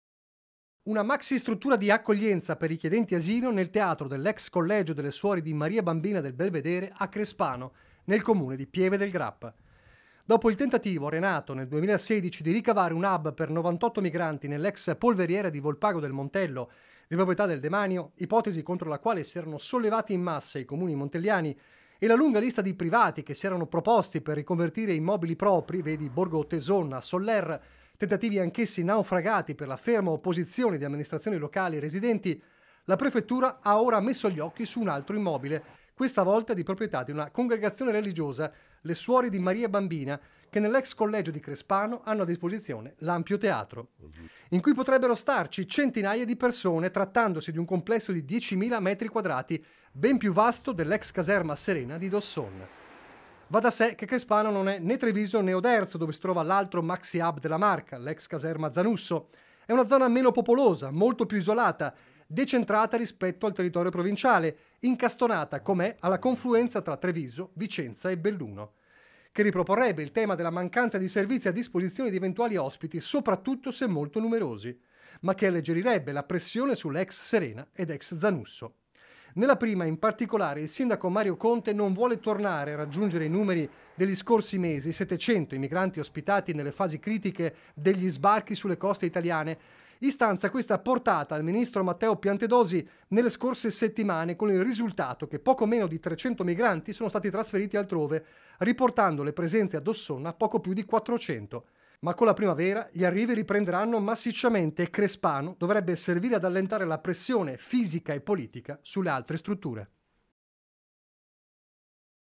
LA TRATTATIVA: UN MAXI HUB PER MIGRANTI A CRESPANO | 12/01/2024
A3 NEWS Treviso 12/01/2024 - PIEVE DEL GRAPPA - Tema migranti in primo piano, perchè in previsione degli sbarchi che torneranno massicci in primavera la Prefettura di Treviso sarebbe in trattativa per creare un grande centro di accoglienza a Crespano (Servizio di Redazione Medianordest) - Segui Antenna Tre anche sul digitale terrestre! Visita il sito